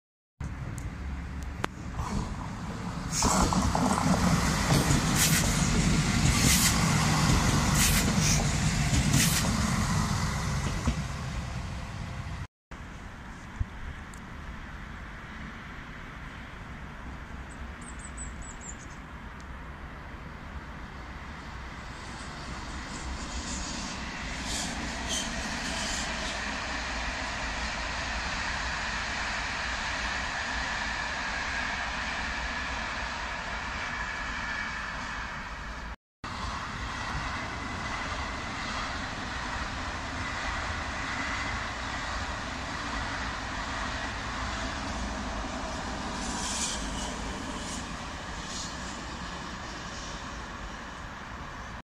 Train en gare d'harfleur Halte
TER et Intercités près de la gare d'harfleur Halte aller voir lucas train 27, Isaac train, Pierre Laurent et france ferroviaire sur facebook à bientôt